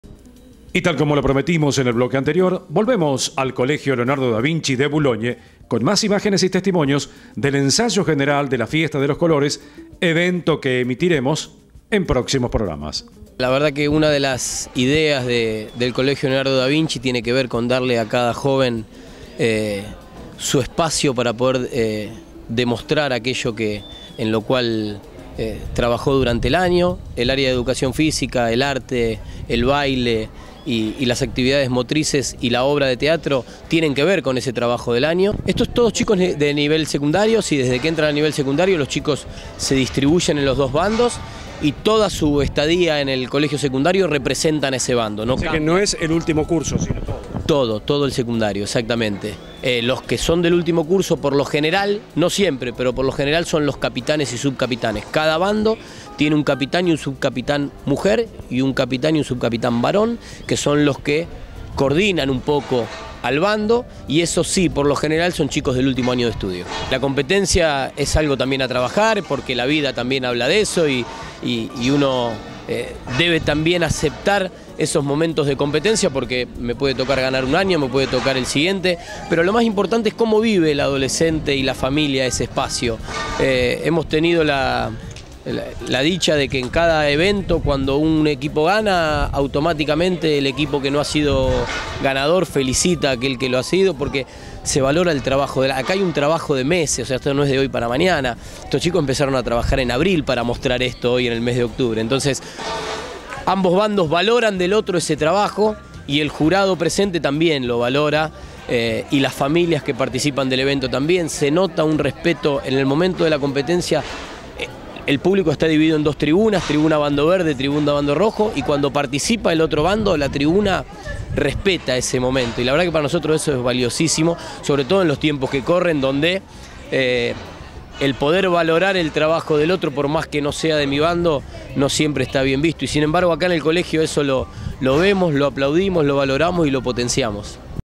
COLEGIO LEONARDO DA VINCI DE BOULOGNE 2018
Colegio Leonardo Da Vinci – Boulogne
Ensayo general de la fiesta de los colores
Y tal como lo prometimos en el bloque anterior, volvemos al Colegio Leonardo Da Vinci de Boulogne con mas imágenes y testimonios del ensayo general de la fiesta de los colores, evento que emitiremos en próximos programas.
PROMOVER 407